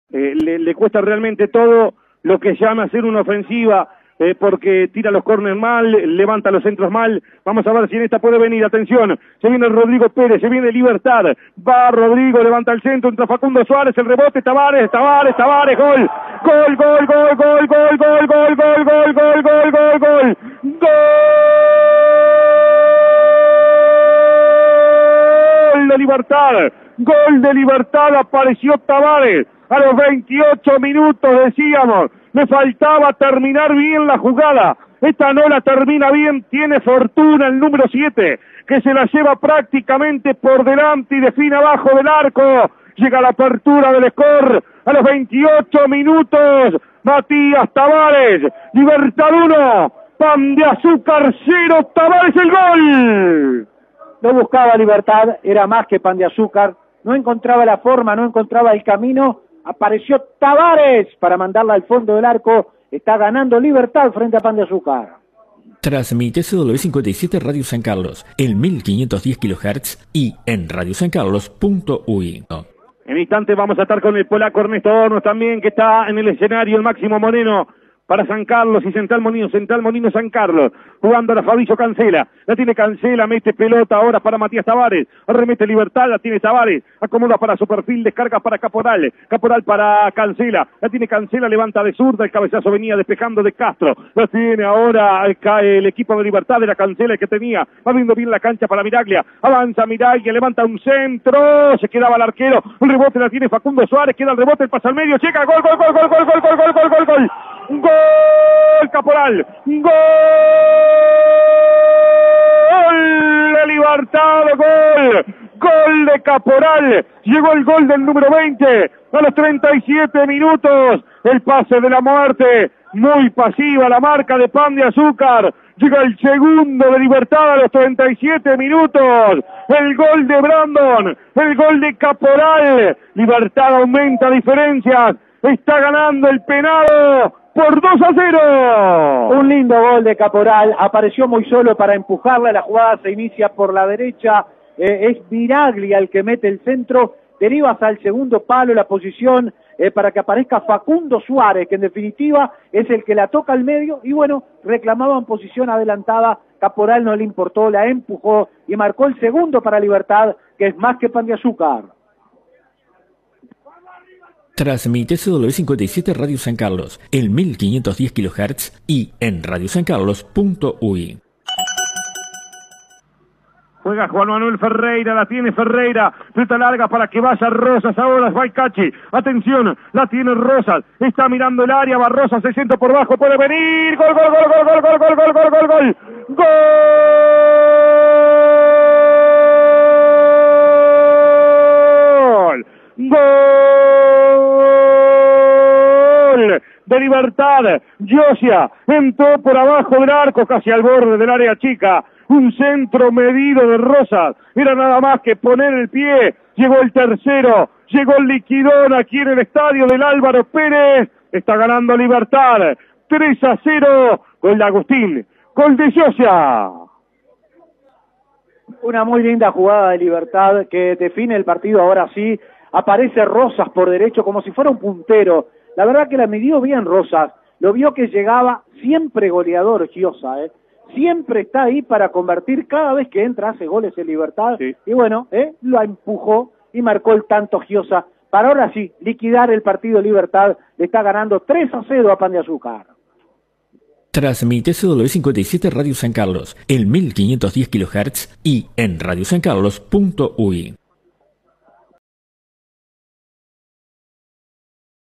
Libertad 3 Pan de Azúcar 0 - Copa Nacional 2022 (por Paul Anderson)
Los goles del partido, narrados por Paul Anderson y comentados por Wilder Benítez para Radio San Carlos.